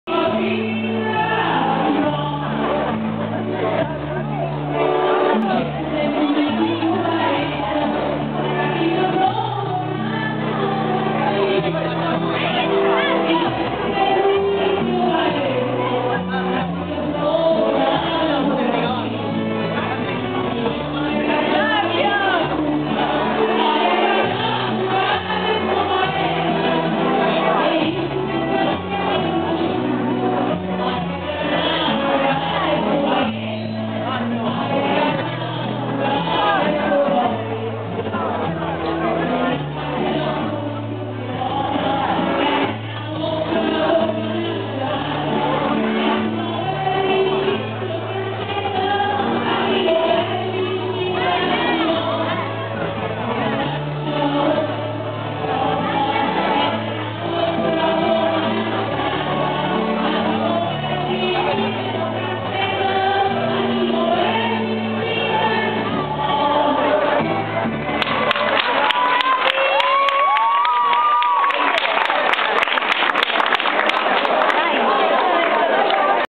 Casoria 02/07/2011 - Traslazione delle reliquie di S. Mauro (06)
"Casoria Ieri & Oggi online" presenta:
Casoria sabato 2 luglio 0re 18,00 Largo S. Mauro organizzato da:
La parrocchia di S. Mauro Abate
"RIEVOCAZIONE STORICA DELLA TRASLAZIONE DELLE RELIQUIE"